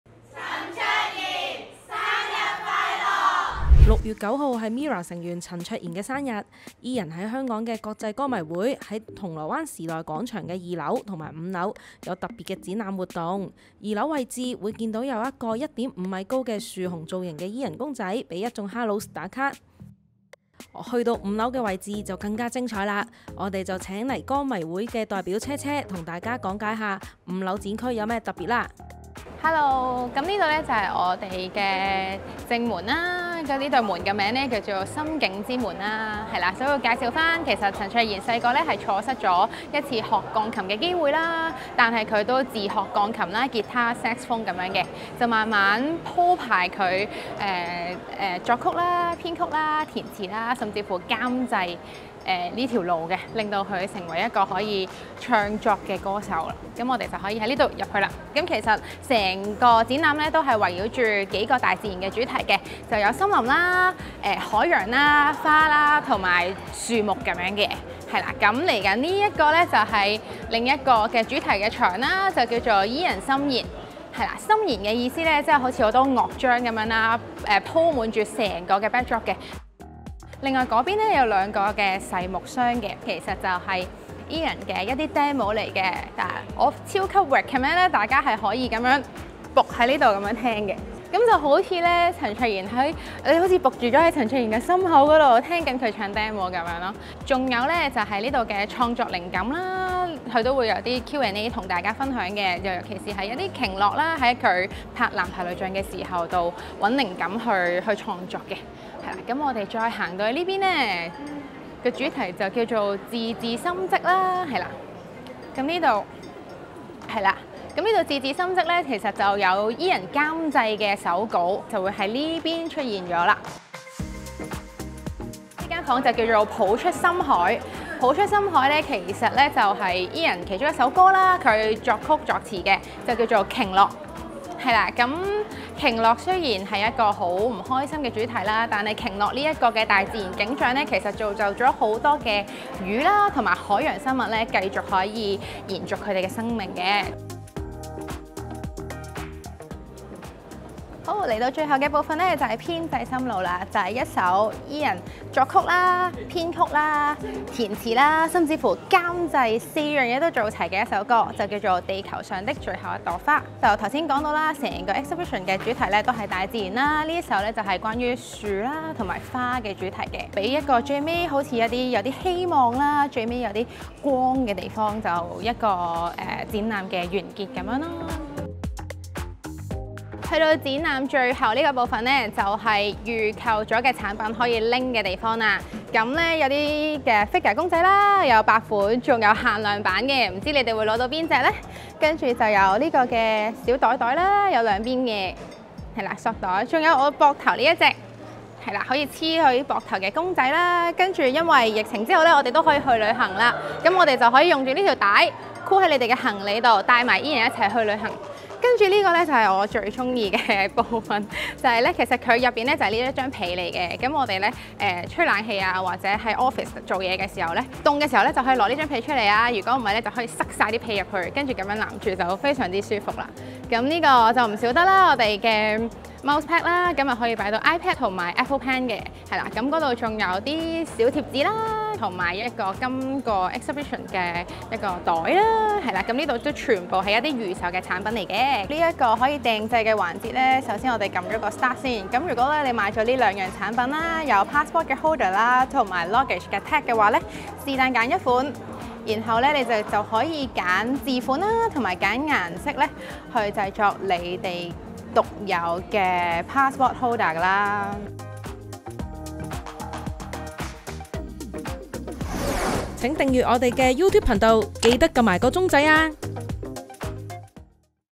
陳卓賢Ian生日應援藝術感超強 多種感官享受鯨魚裝置最高質
MIRROR成員陳卓賢將於下月9日迎來30歲生日，陳卓賢國際歌迷會（香港）去年在九展舉行盛大展覽會，今年移師到銅鑼灣時代廣場2樓及5樓舉行「伊人森弦@Ian's Asteroid I609」展覽，由即日起到下月30日，大家可以看到Ian的創作手稿和歌曲Demo，而今次展覽會的藝術成份非常高，令人如置身藝術館的感覺。

bmgproductionmusic